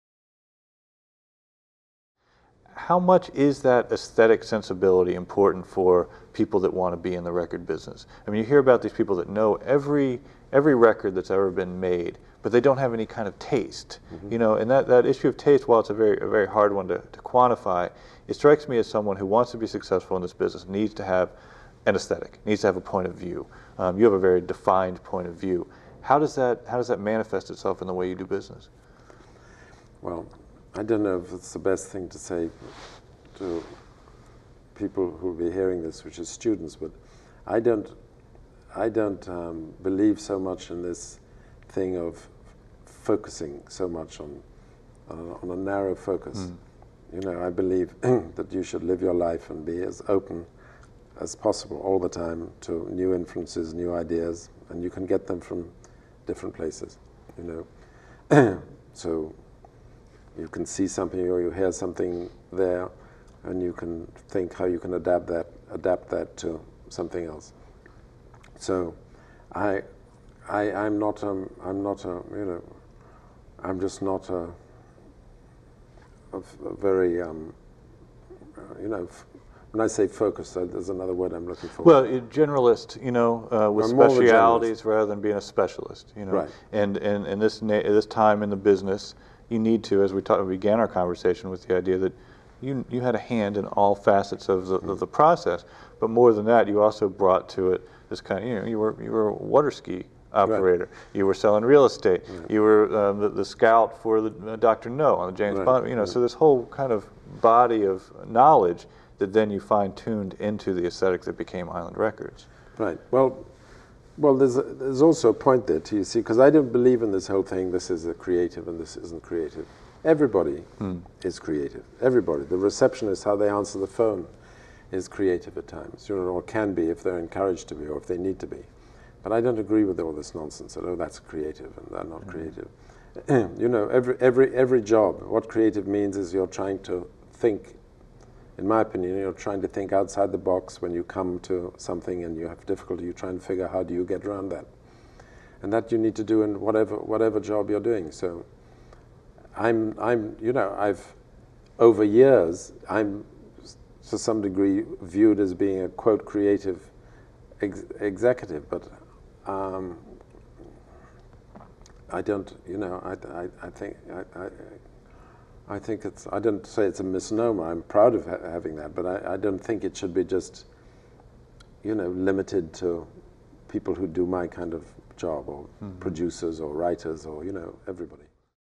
Interview - Chris Blackwell on Musical Taste and Success
Chris Blackwell, founder of Island Records, talks about the importance of taste and openness to new things to professionals operating at any level of the music industry. Presented by Loyola University New Orleans.